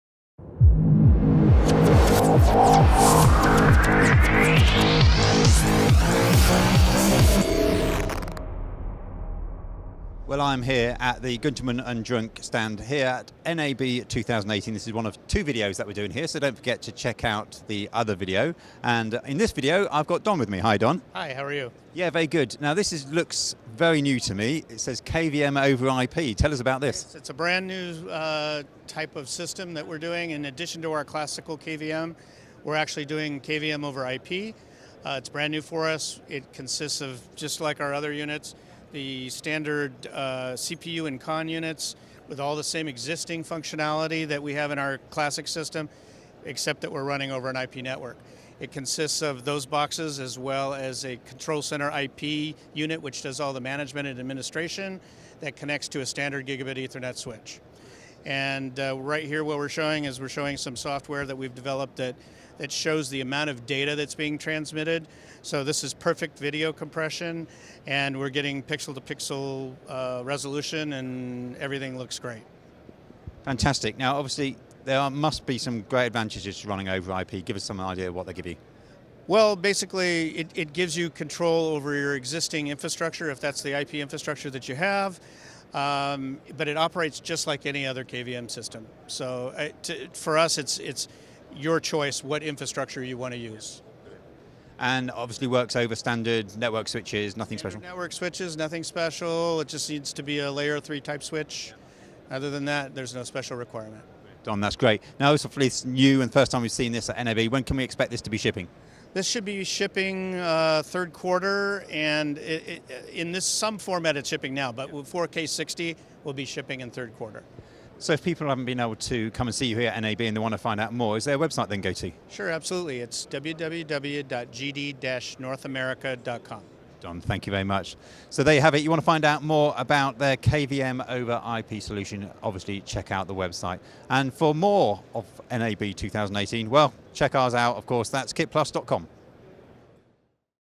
KVM over IP from Guntermann & Drunck at NAB 2018
Don explains KVM over IP direct from NAB 2018 - Las Vegas